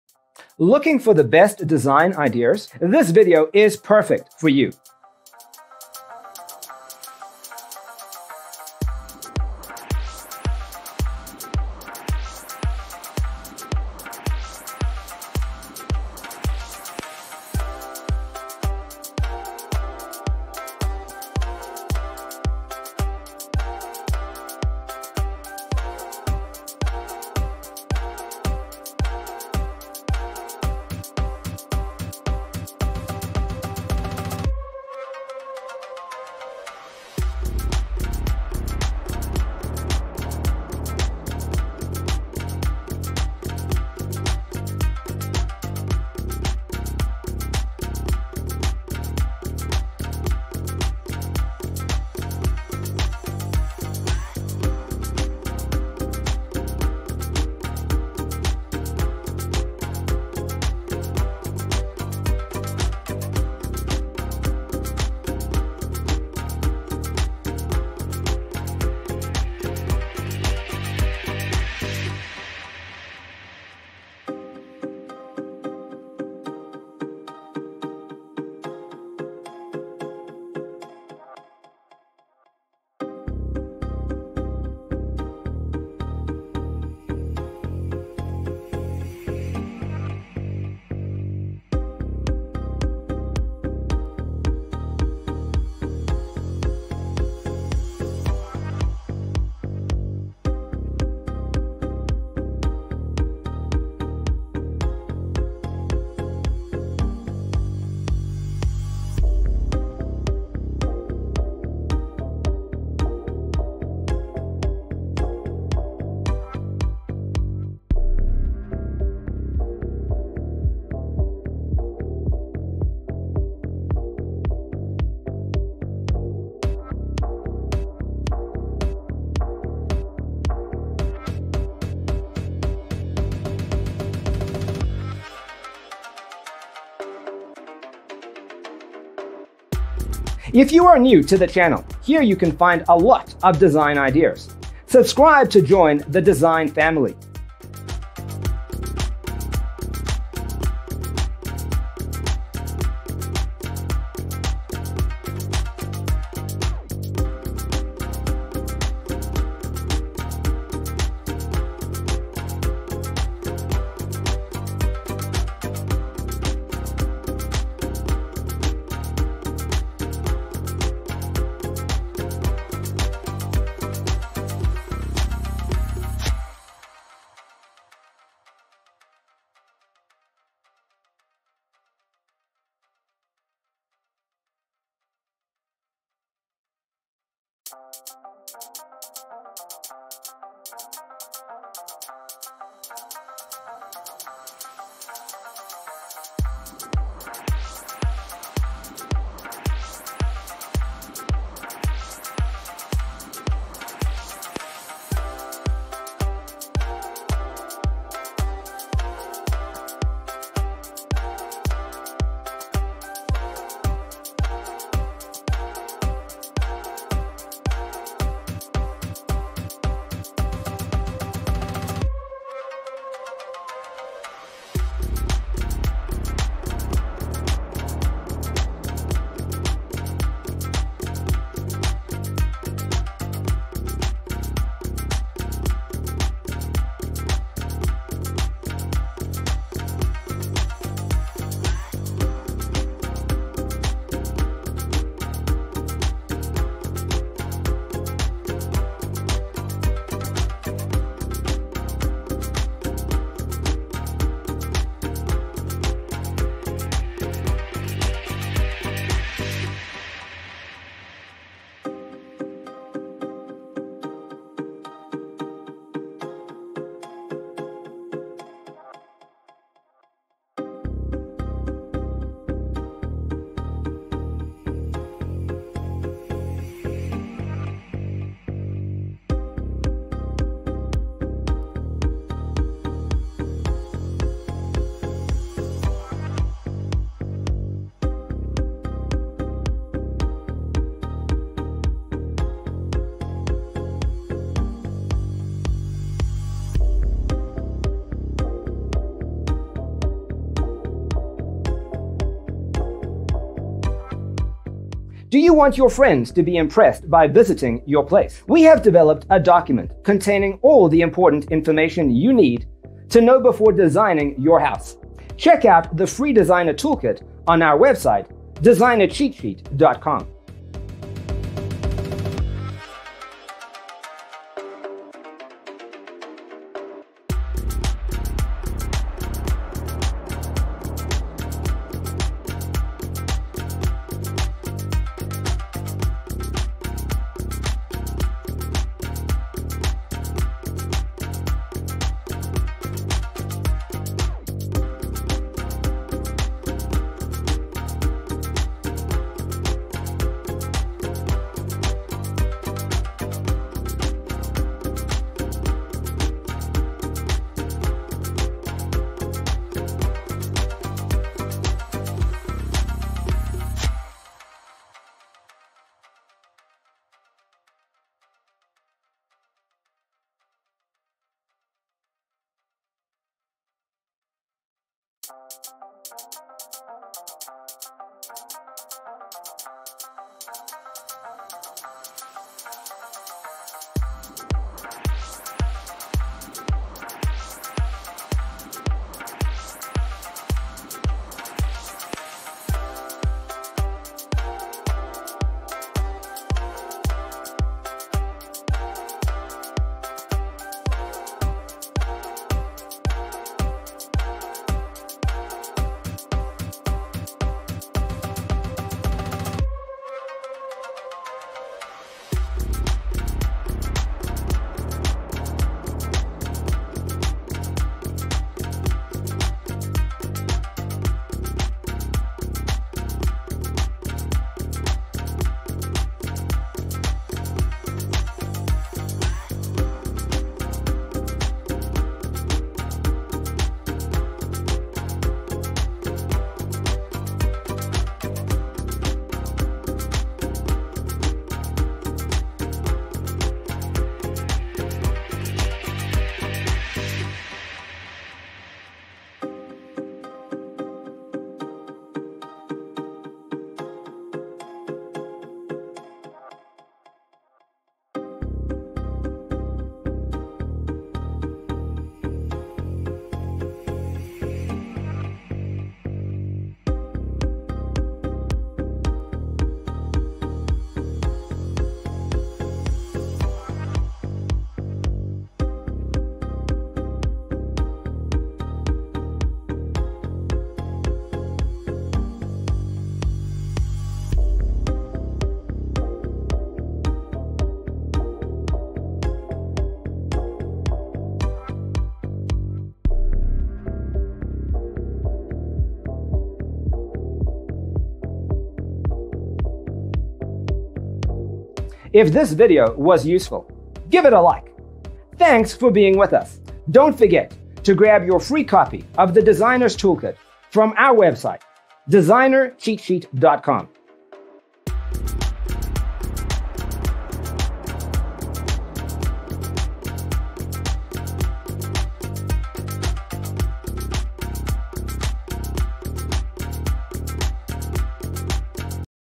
75 Budget Craftsman Living Space Design Ideas You'll Love 🌈
Browse craftsman living room and family room ideas. Discover design inspiration from a variety of craftsman living spaces, including home theaters, sunrooms and more.

0:00:00 - Intro
0:00:12 - Budget Craftsman Living Space Design Ideas You'll Love: Inspirations 1 to 25
0:03:03 - Budget Craftsman Living Space Design Ideas You'll Love: Inspirations 26 to 50
0:06:07 - Budget Craftsman Living Space Design Ideas You'll Love: Inspirations 51 to 75

Hello there!🌈